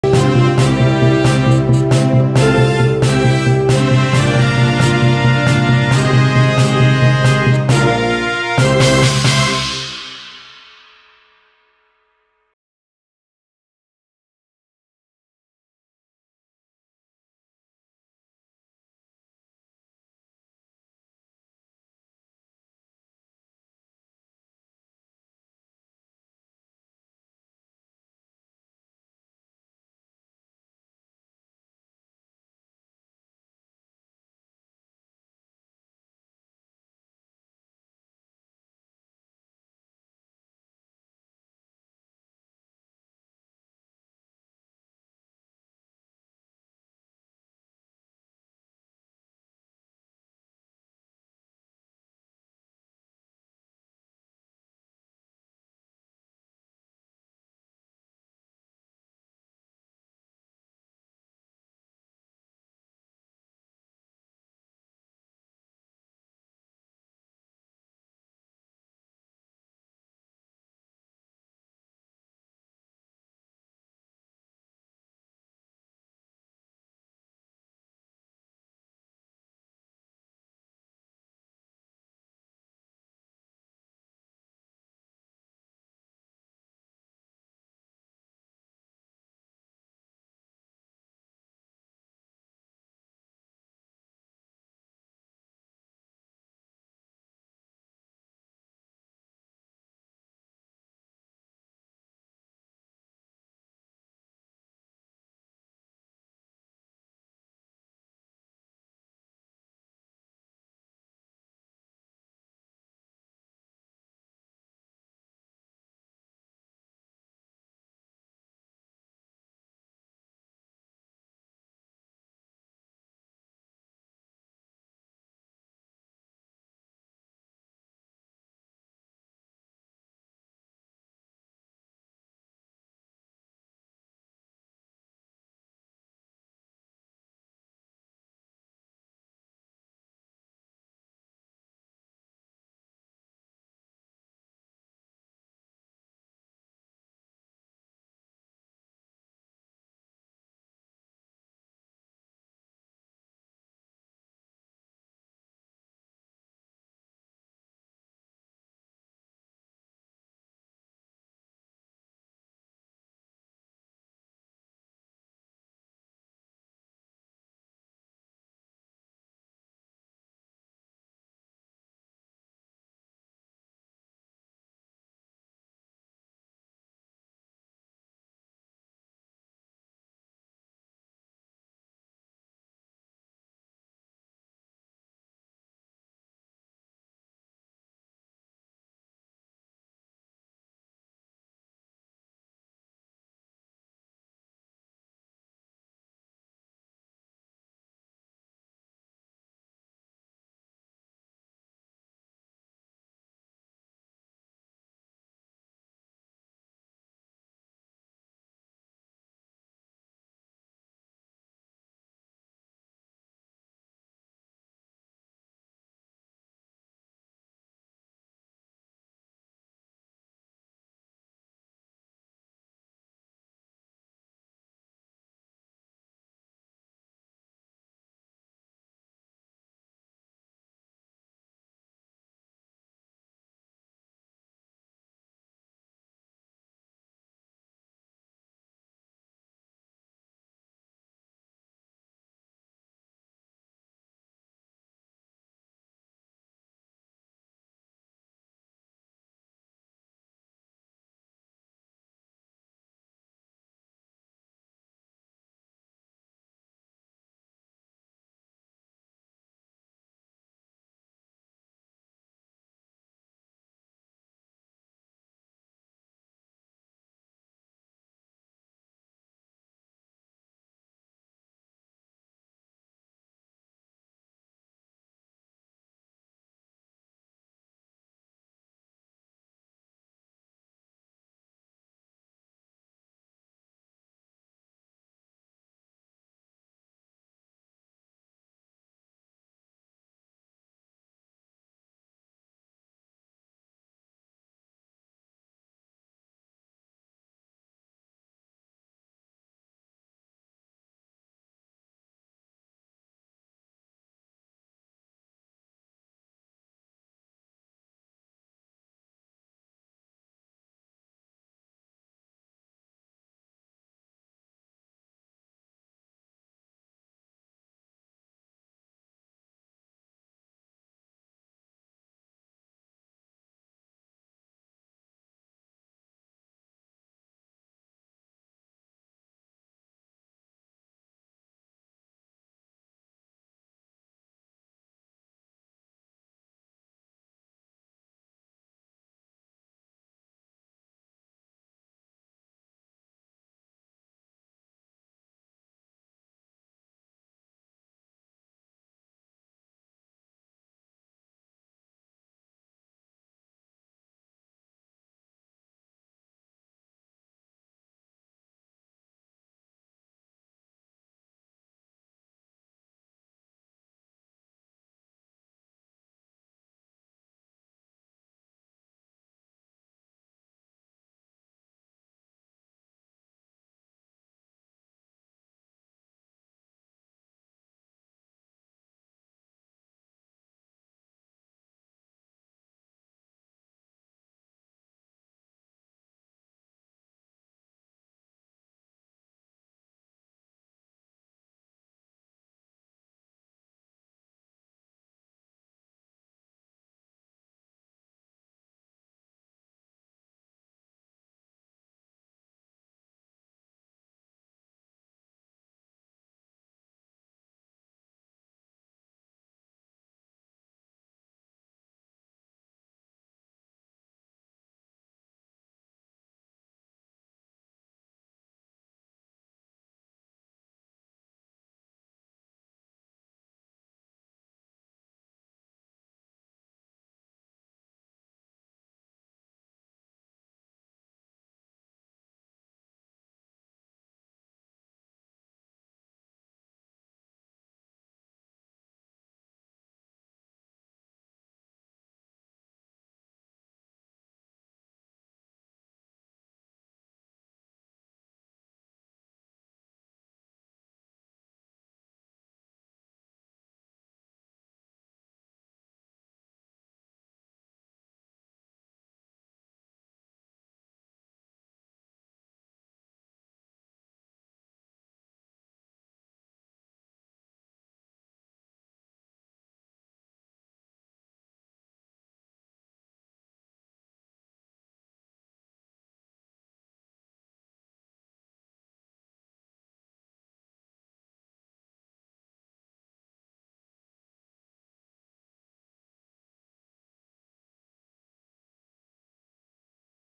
BONNIEGAMER110 BREAKING NEWS!
Some Great News In This Video Me And My Friends on Roblox Are Going To Make A. ROBLOX GAME! Its gonna be a fnaf RP but with our OC's and fanmade maps of what we think a fangame about our OC's Would Look Like Comment Below YAY! if you hyped or NAH if ya hate the idea.